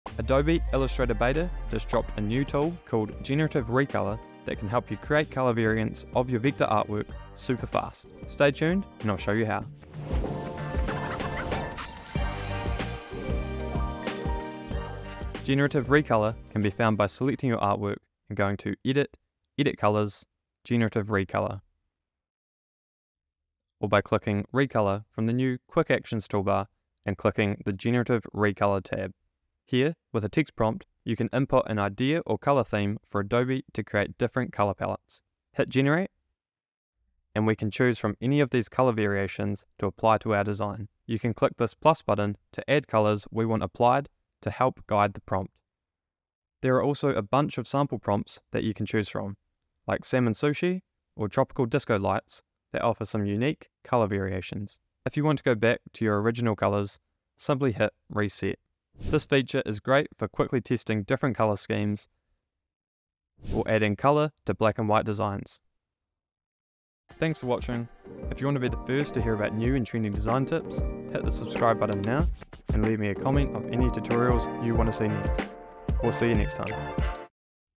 How to Instantly Re-color your Designs
Produce Color Variants super fast with Adobe Illustrator's New AI Text-Prompt Generative Recolor Tool!

Generative Recolor Beta version is now available in both Adobe Illustrator and Adobe Illustrator Beta.